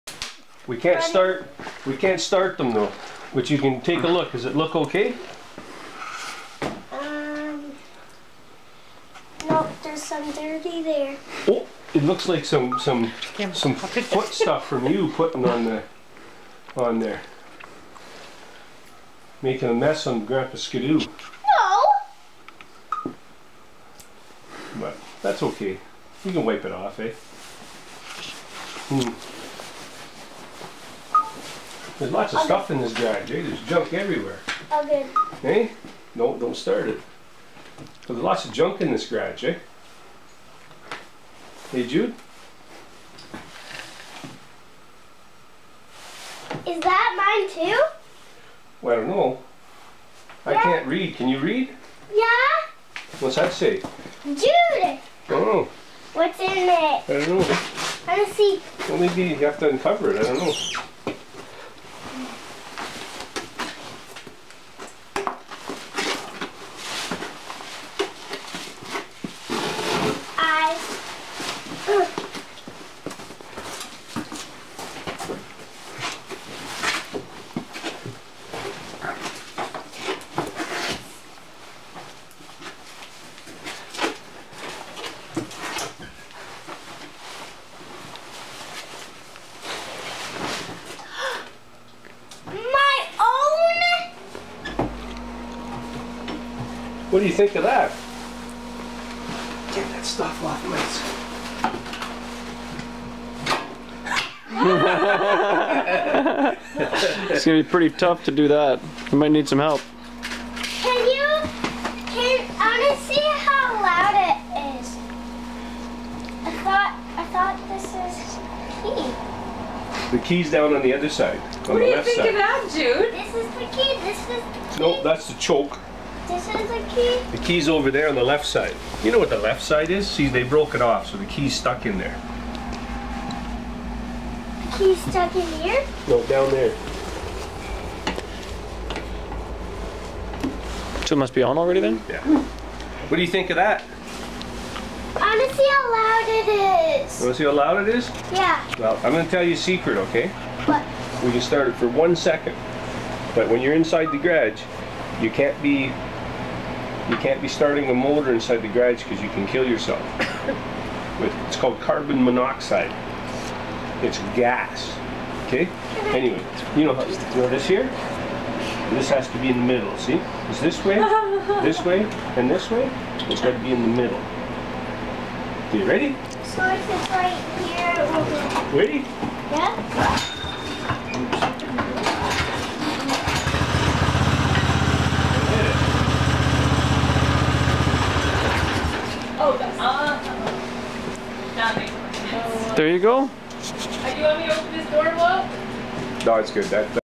First Ski-Doo
Jude gets his first Ski-Doo